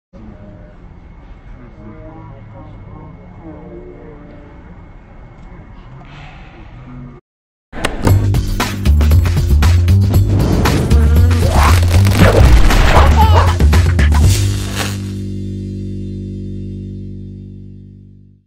Golden Retriever decides to ride baggage carousel at airport
If you look closely on the far side of the baggage carousel, you can see a very good boy waiting for his family to take him home from his arrival at El Dorado Airport! Hilarious!